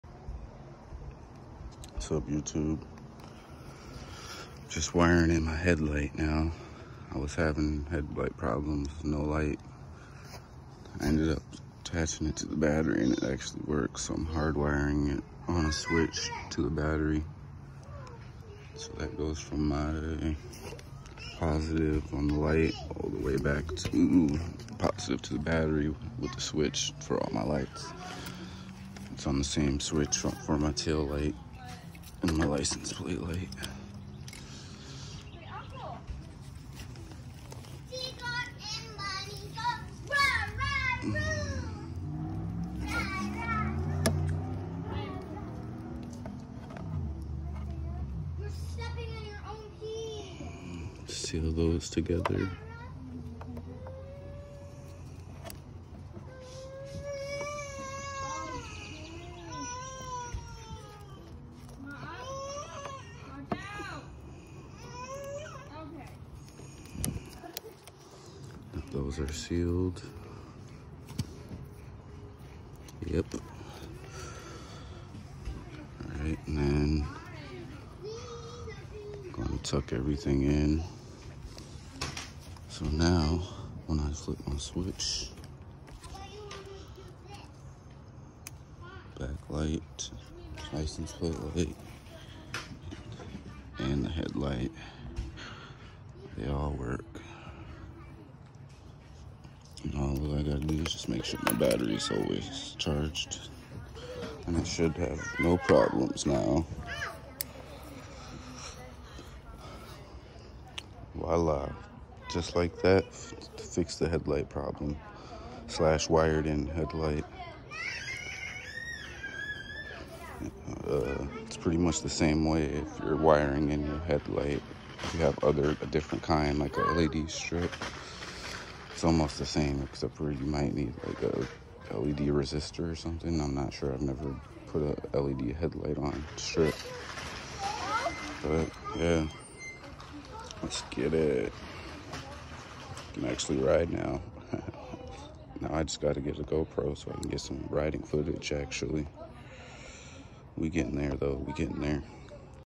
Pit bike Pocket Bike headlight add on
Had to do a quick fix on my street legal mini bike.

MotoZilla Rich car and motorcycle reviews, finds, riding.